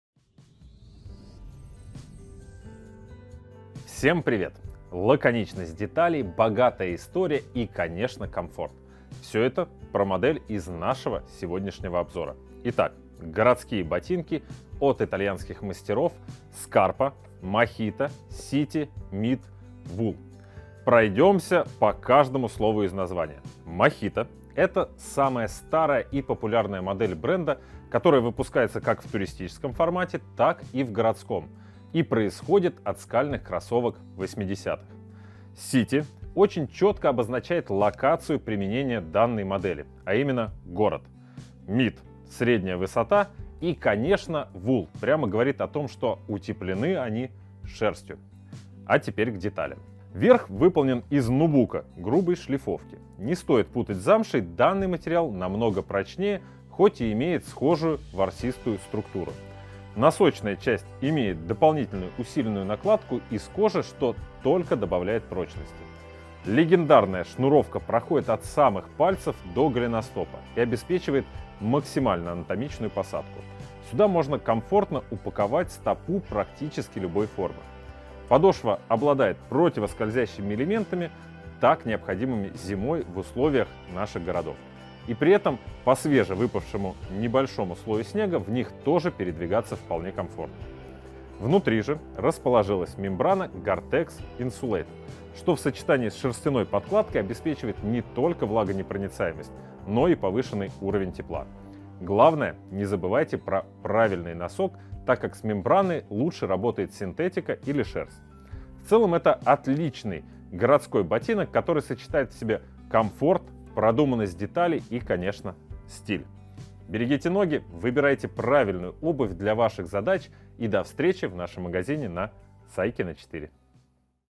Ботинки Scarpa Mojito City Mid Gtx Wool. Обзор
Стильные, удобные и очень комфортные ботинки!
Лаконичный дизайн сочетается с новейшими технологиями, которые позволяют с максимальным удовольствием носить Mojito City в течение всего дня, сохраняя при этом ноги в тепле и сухости.
Традиционная для обуви Scarpa глубокая шнуровка, помогает достичь максимально-анатомичного прилегания с учётом особенностей именно вашей стопы.
Специальные антигололёдные вставки в подошве дают чувство уверенности на сложных зимних поверхностях.
Scarpa Mojito City Mid Gtx – в этой обуви не холодно на улице и не жарко в помещении.